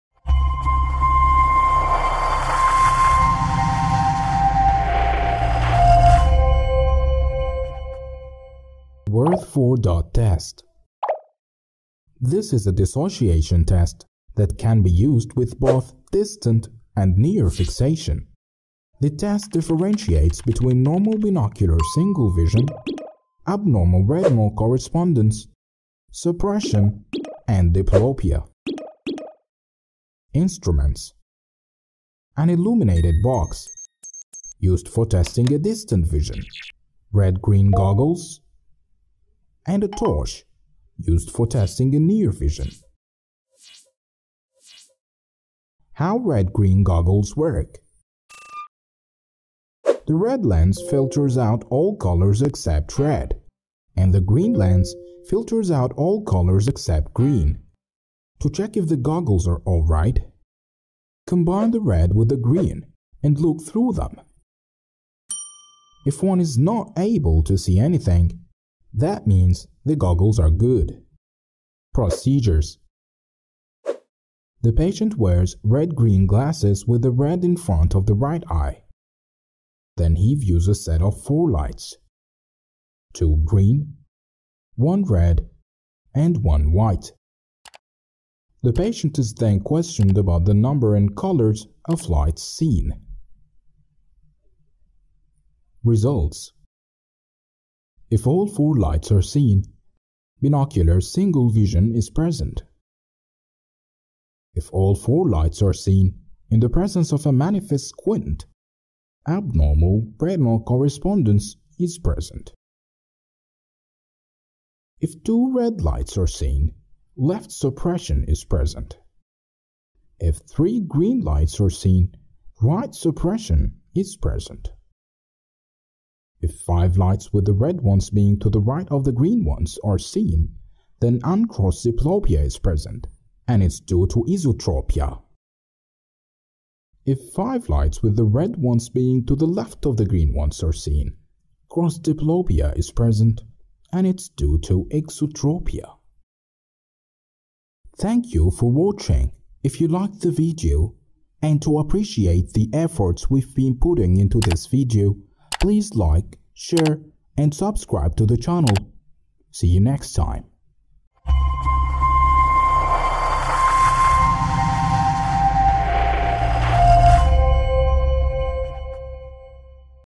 Worth's Four Dot test -Sudanese Optometry Portal - بوابة البصريات السودانية
Dr. Layali Ibrahim Hassan
Worth's 4-Dot test is simple and quick method used for investigating diplopia, suppression, fusion, and anomalous retinal correspondence.